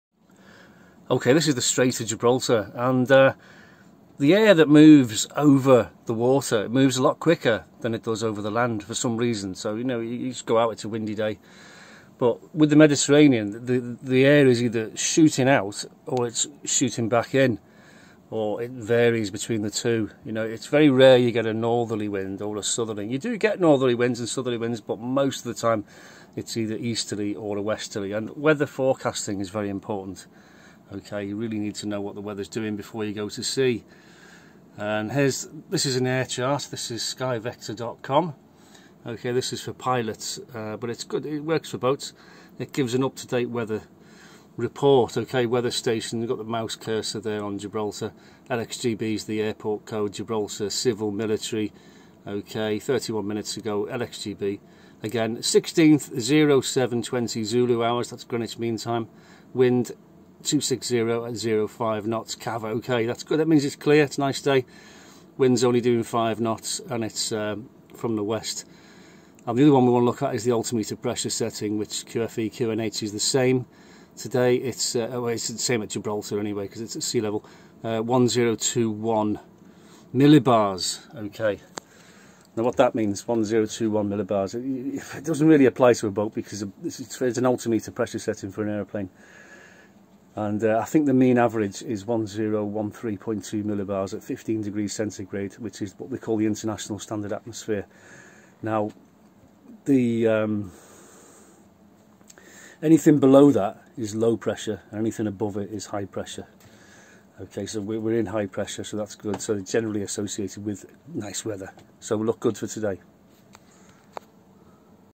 Notoriously dodgy waters.
Weather in the Strait of Gibraltar can be very dodgy indeed.